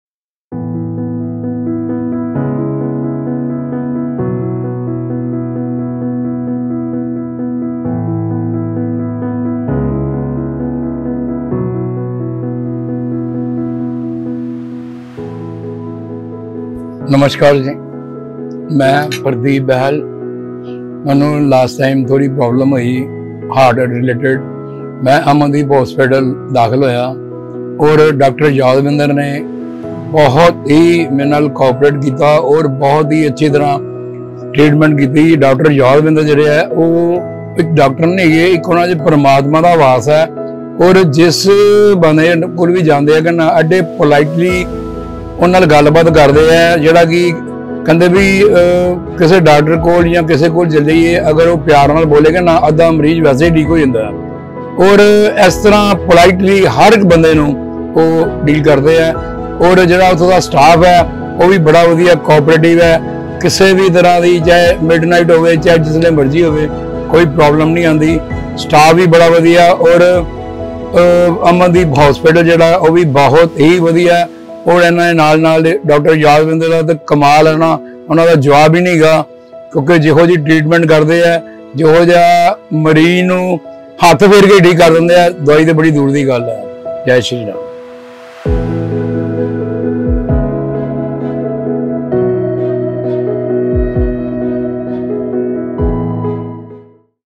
RECOVERY FROM A MAJOR HEART ATTACK | CARDIO | DR YADVINDER SINGH | AMANDEEP HOSPITAL
In this inspiring video, we bring you the heartwarming story of Mr. Pardeep Bahal, a patient who successfully recovered from a major heart attack. Mr. Bahal shares his experience of how Dr. Yadvinder and the Cardio team from Amandeep Medicity Hospital in Punjab, India, worked tirelessly to save his life.

Through Mr. Bahal's personal account, you'll gain insight into the level of care and expertise provided by the medical staff at Amandeep Medicity Hospital. His story serves as a testament to the hospital's commitment to excellence in patient care, as well as the compassion and dedication of its doctors and healthcare professionals.

If you or a loved one are facing heart-related health issues, this video will give you hope and inspire you to take action towards a healthier life. Watch now and discover Mr. Bahal's road to recovery and how Amandeep Medicity Hospital played a crucial role in his journey towards a healthier and happier life.